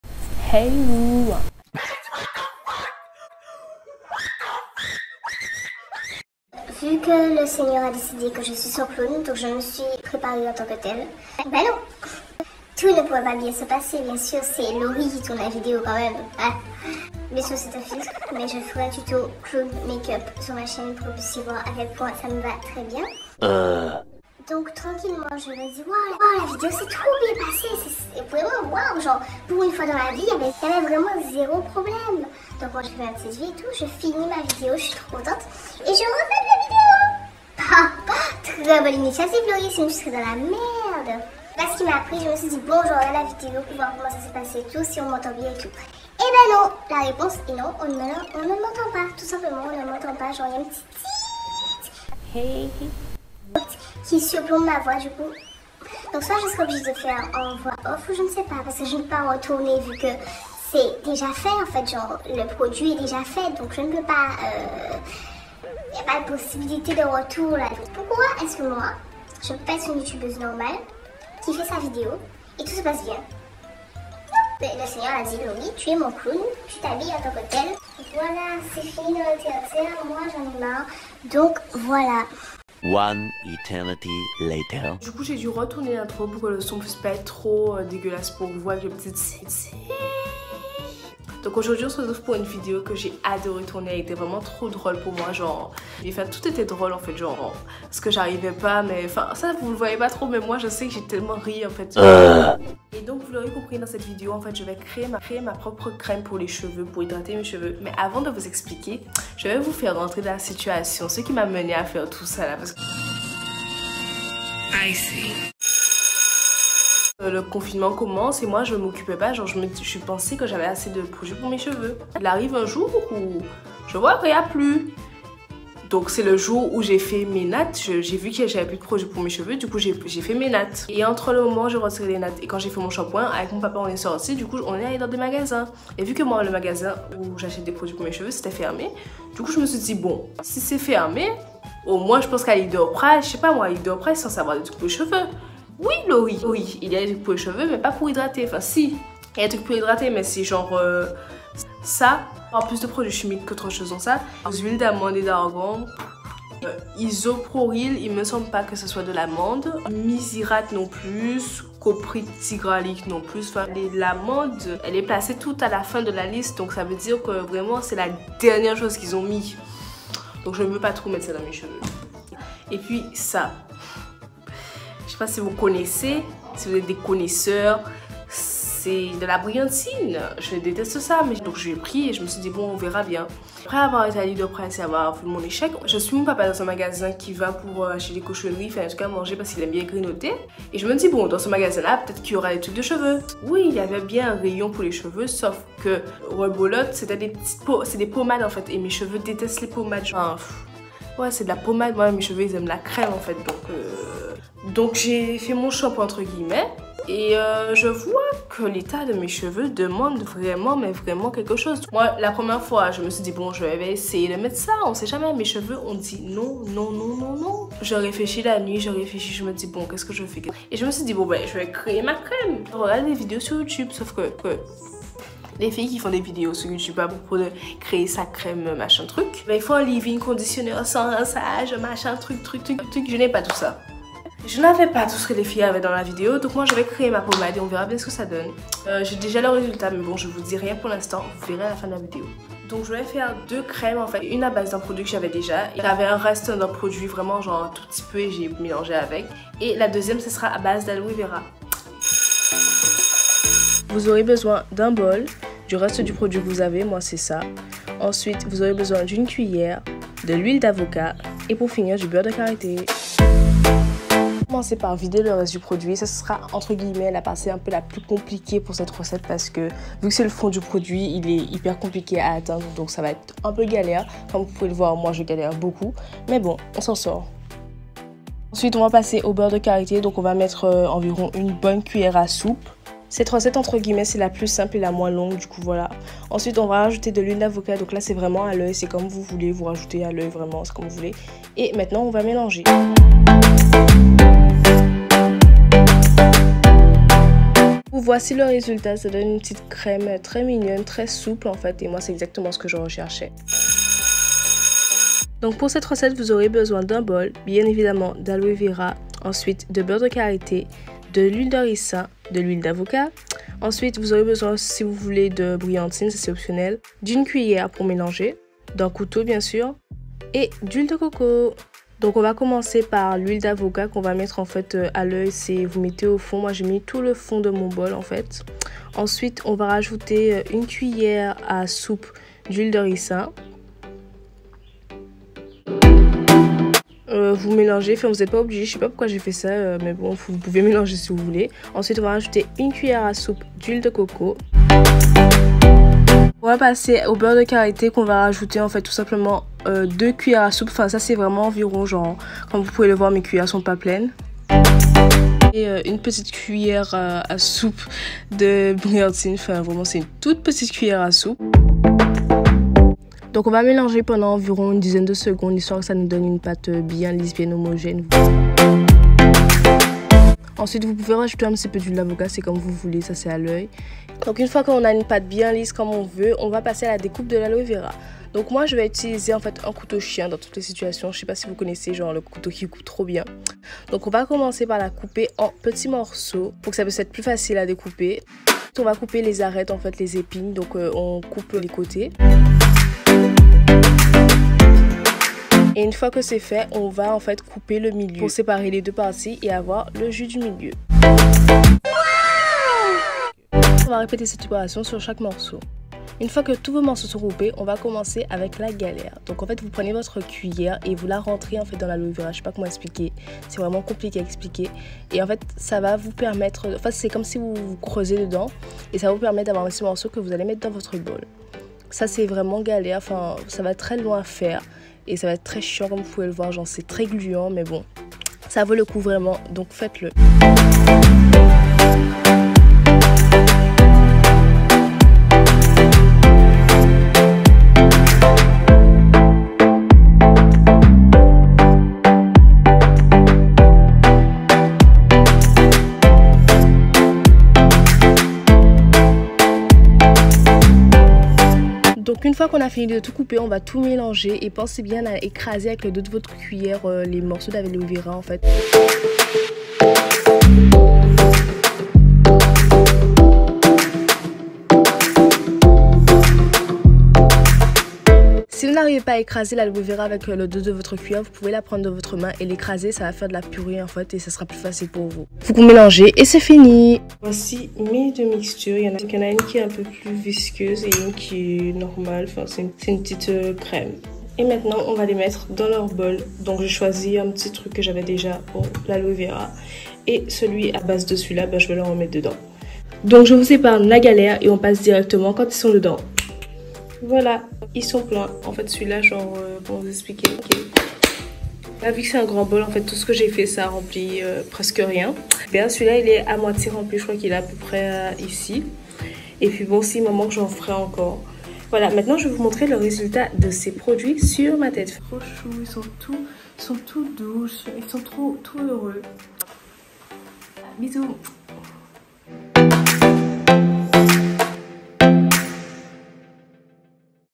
CRÈME HYDRATANTE POUR LES CHEVEUX FAITE MAISON || laury mj
Salut vous!!
Ces recettes sortent droit  de mon imagination, J’espère que la vidéo vous plaira ça m’a beaucoup amusé de faire ça. Et j’espère que si vous êtes dans la même situation que moi cette recette vous aidera.